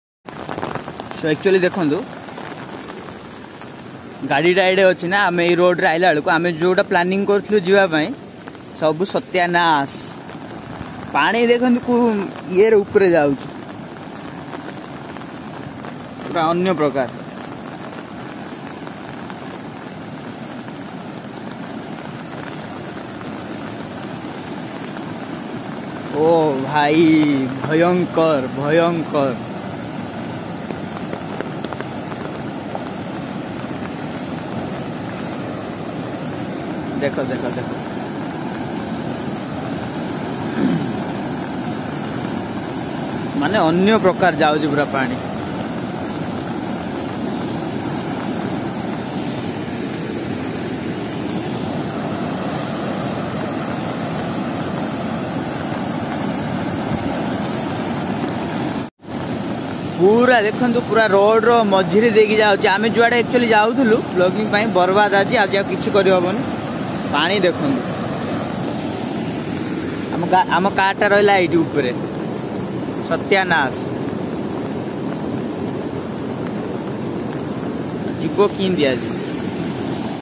1st ବନ୍ୟା ପରିସ୍ଥିତି ଓଡ଼ିଶାରେ! ରୋଡ ଉପରେ ବନ୍ୟା ପାଣି!😰 1st flood in Odisha.
this is just a few minutes video on heavy rain in ODISHA .